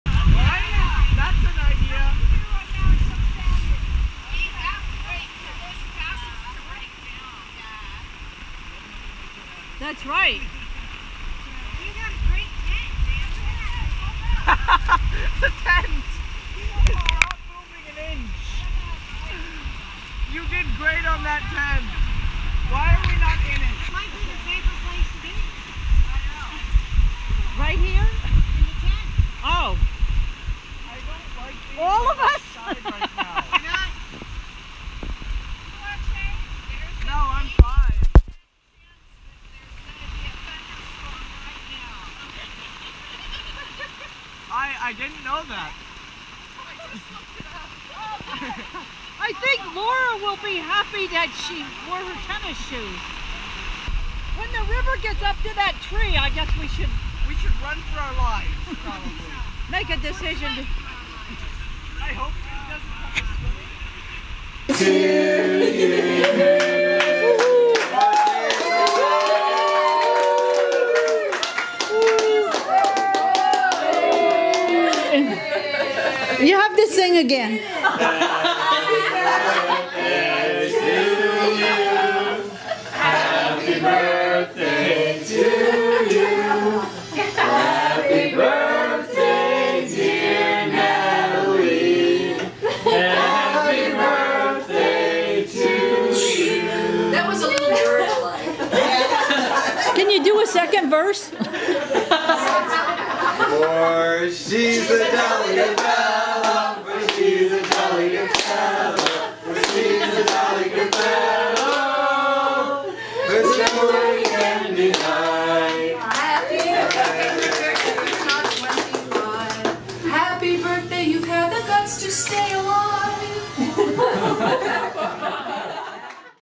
Natalie's Birthday Party at Cooper's Landing!
Natalie had a party and a tornado almost came!